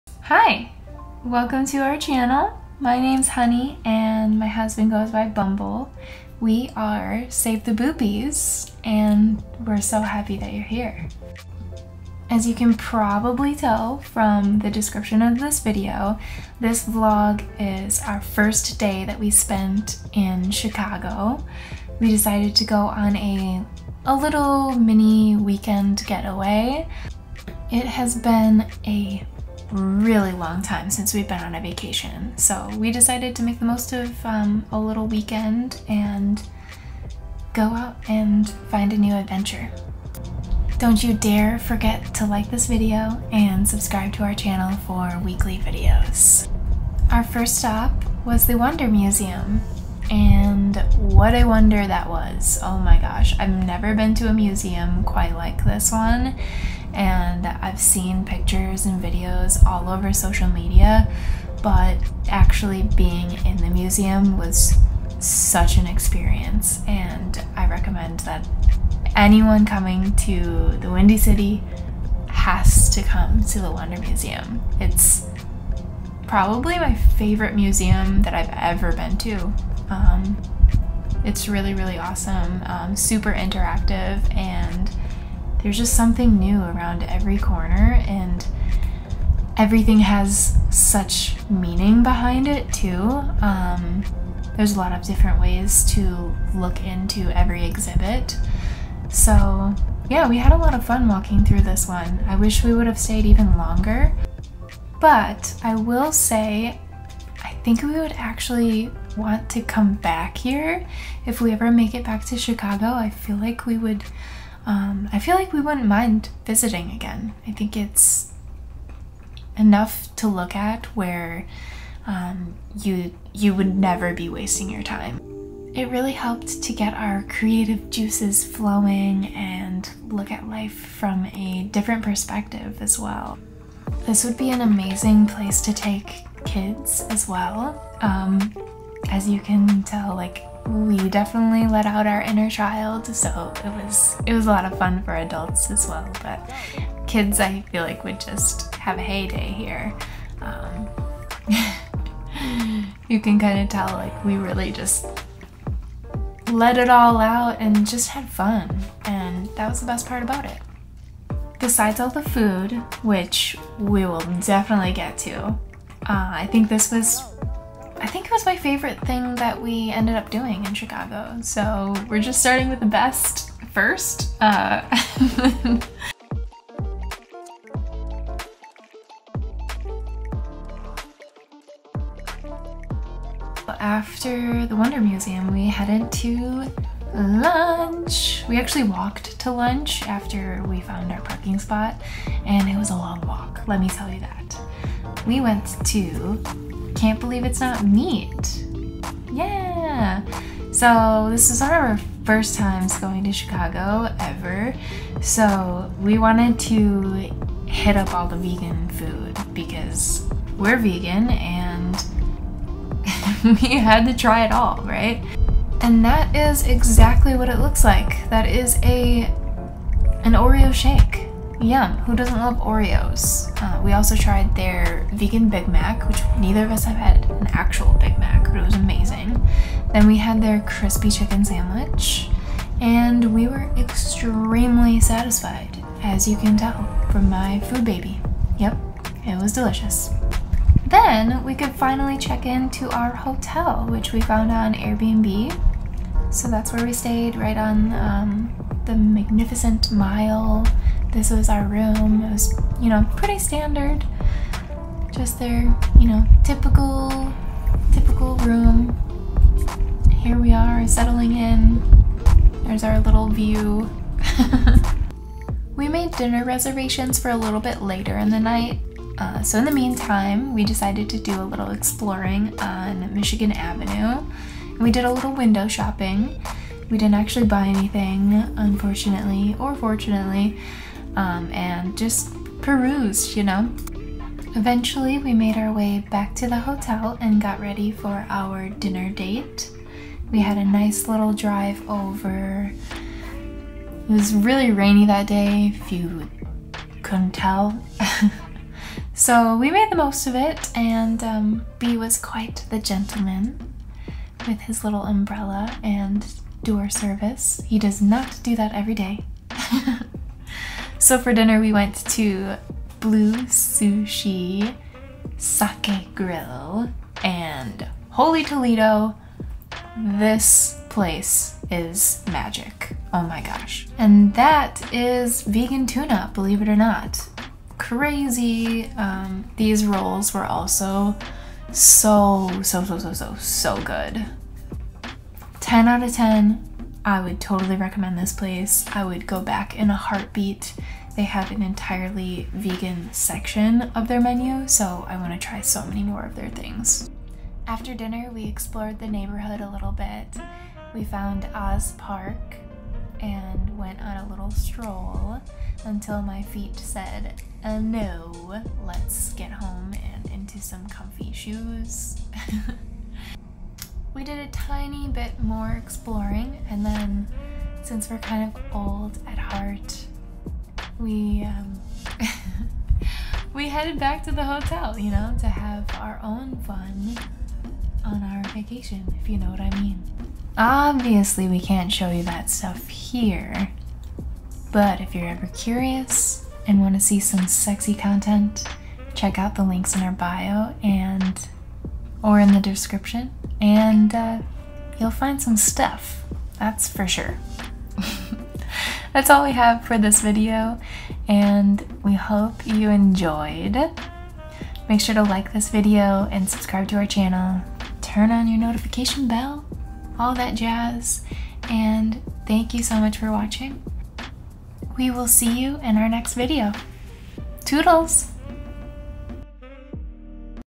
Chicago Travel Vlog | Day 1 Exploring & Eating🌱
Hi! We're Honey & Bumble...AKA the Bees. Enjoy these snippets of our first day exploring the Windy City!

let's bee friends!

To Enter:
🤍Like our very first video
🤍Comment a question for us under our first video
🤍Follow @savehoneybee on IG
🤍Follow @savetheb00bees on Twitter